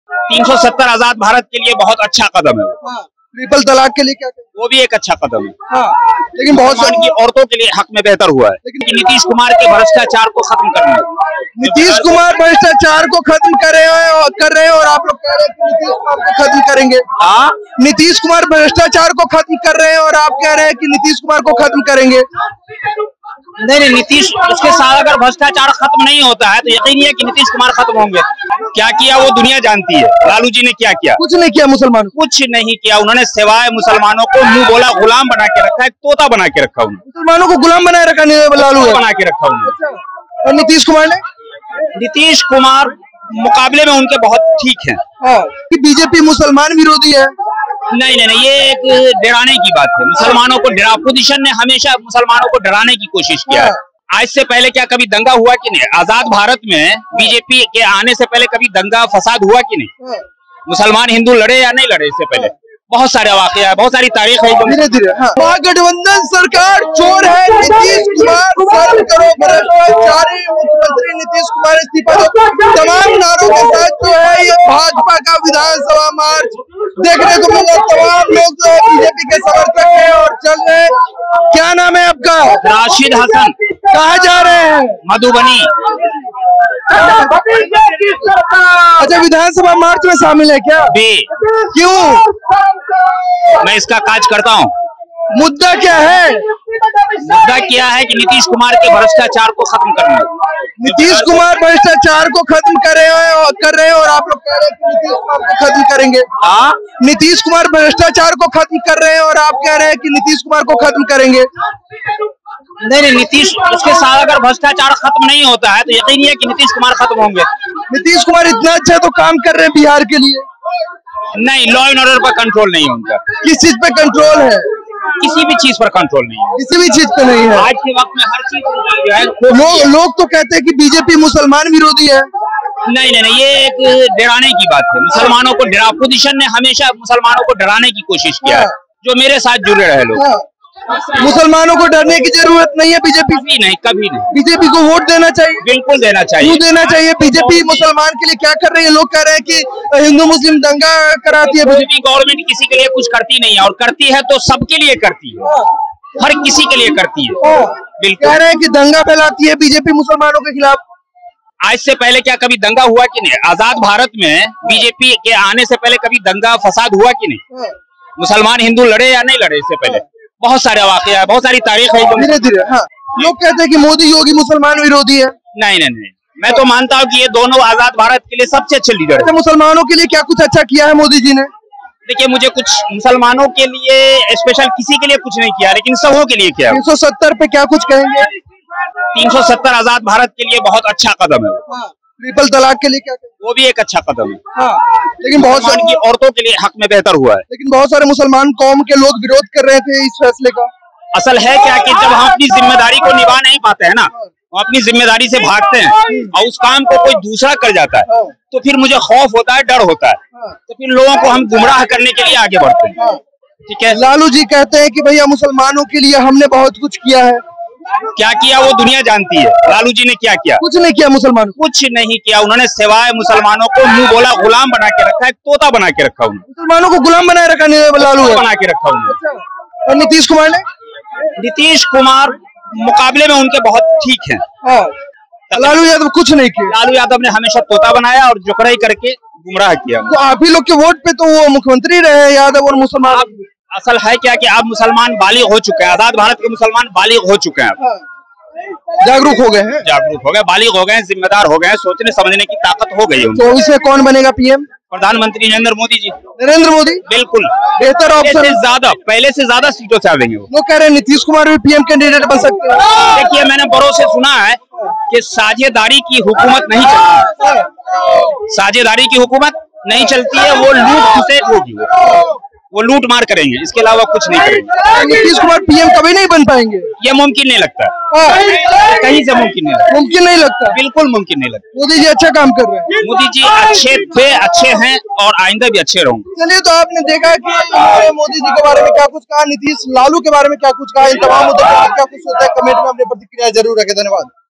BJP के आंदोलन में पहुंचे मुसलमान, बोल रहे कि इस Nitish-Tejashwi सरकार को उखाड़ कर फेंक देंगे!
भारतीय जनता पार्टी का साथ देने के लिए मुसलमान चले आए हैं और कह रहे हैं कि इस बिहार सरकार को उखाड़ कर फेंक देंगे! देख लीजिए कैसे गरज रहे हैं!